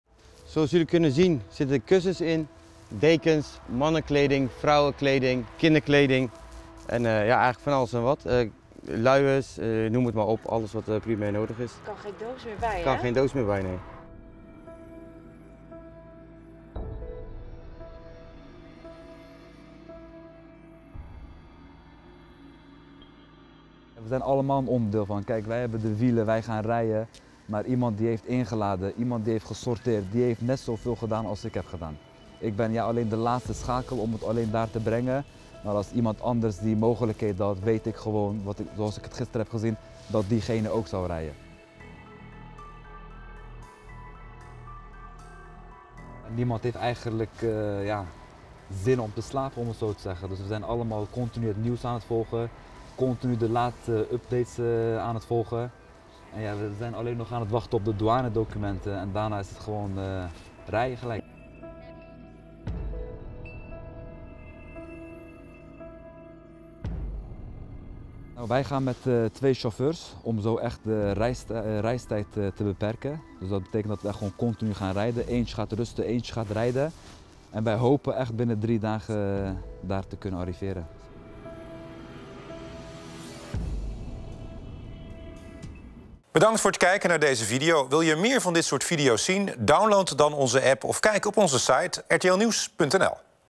Met hulpgoederen in één ruk van Zwijndrecht naar Turkije
Twee vrachtwagens vol hulpgoederen uit Den Haag en omgeving vertrekken vandaag naar Turkije. Chauffeur Nevzat en zijn collega kunnen niet wachten om hulp te bieden in het getroffen gebied. "We wachten alleen nog op de douanedocumenten en dan gaan we gelijk rijden."